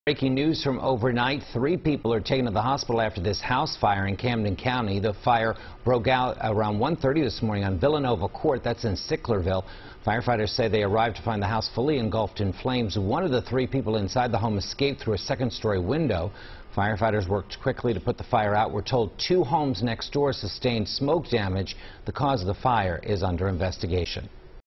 At least 8 displaced by Sicklerville house fire
Three people were taken to the hospital after a house fire in Camden County.